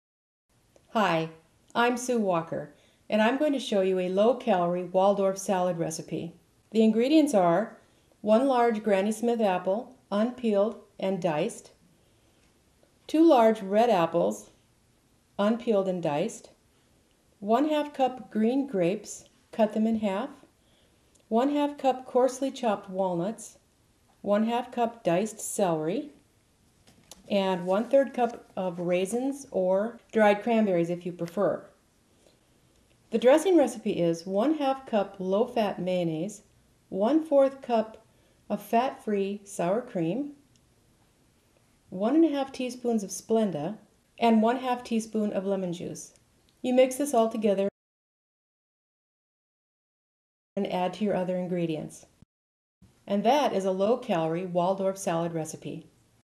Salad Recipes : Low-Calorie Waldorf Salad Recipe
A Waldorf salad can be low-calorie if you use the right ingredients when making your dressing. Enjoy a low-calorie Waldorf salad with help from a head cook in this free video on salad.

Expert: Susan Walker
Bio: Sue Walker owns her own freelance cooking business. She worked as a pastry chef and a full-time head cook for more than 10 years before deciding to start her own company.